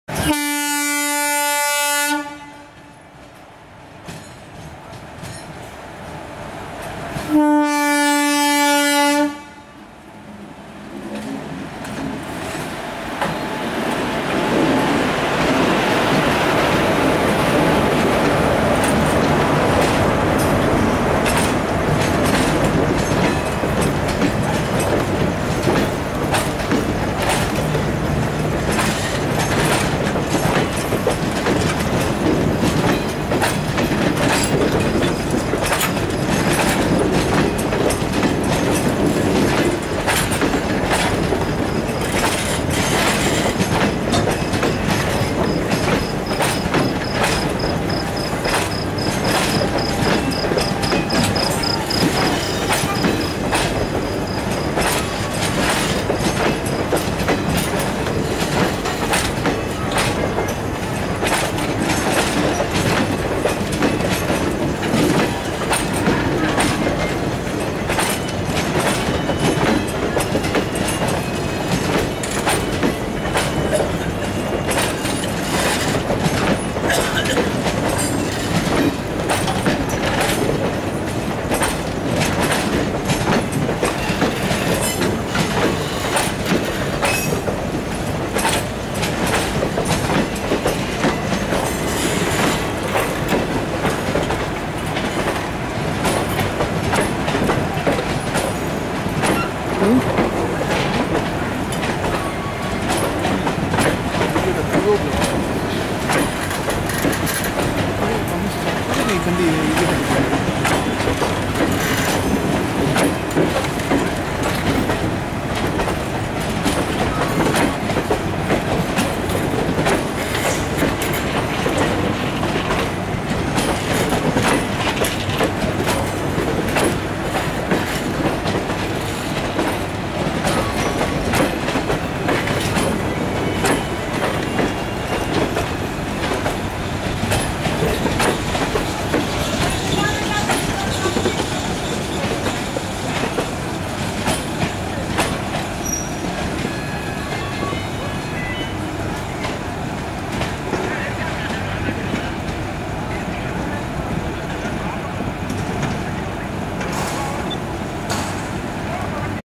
Blood red RPM WAP-4 Mumbai Chennai Mail completes journey, and makes a grand arrival at Chennai
At the crack of dawn in the month of November, beautiful blood red 22785 RPM WAP- 4 makes a smart entry at Chennai Central station with the before time running 11027 Mumbai C.S.T. (CSTM)- Chennai Central (MAS) MAIL.

The MAIL follows the Mumbai CST- Kalyan- Karjat- Pune- Daund- Kurdavadi- Solapur- Hotgi- Gulbarga- Wadi- Raichur- Adoni- Guntakal- Gooty- Kadapa- Renigunta- Arakkonam- Chennai route.